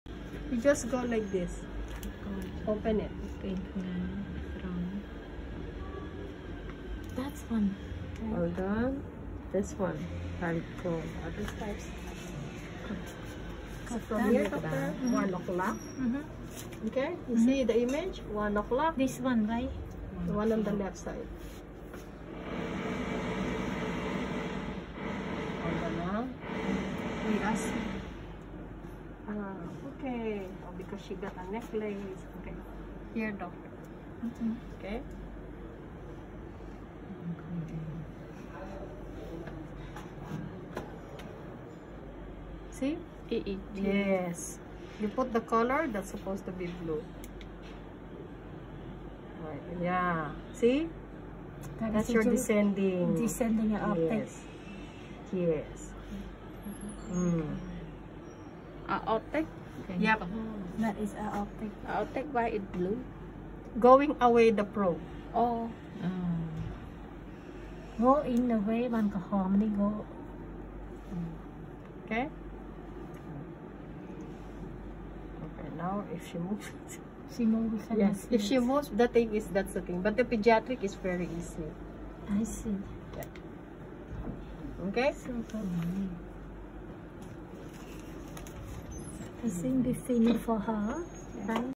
Video 13: Echocardiography /heart ultrasound /2D
How to measure heart by ultrasound